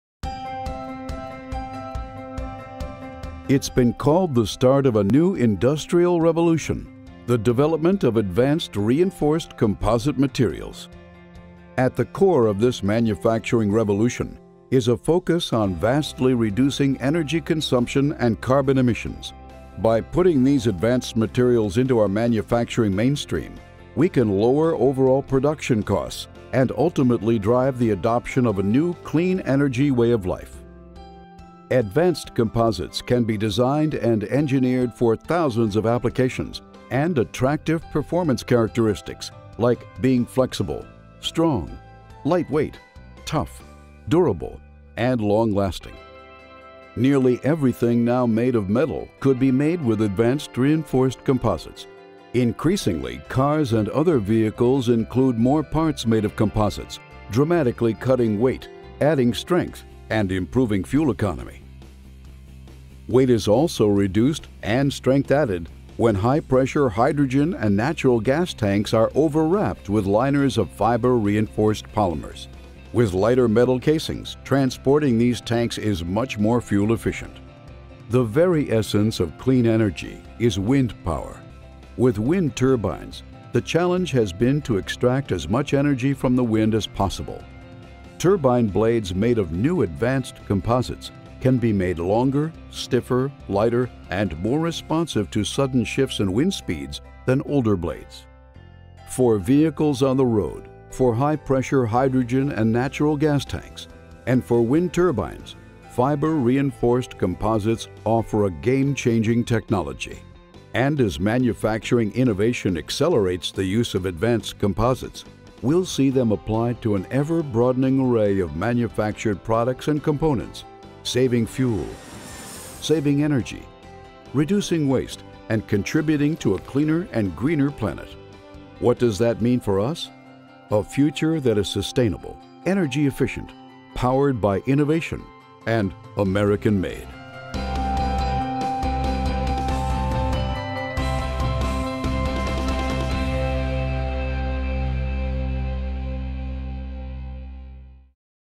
Advanced Manufacturing: Composites & Clean Energy
Advanced fiber-reinforced polymer composites, which combine strong fibers with tough plastics, are lighter and stronger than steel.  These materials could lower overall production costs in U.S. manufacturing and ultimately drive the adoption of a new clean energy way of life.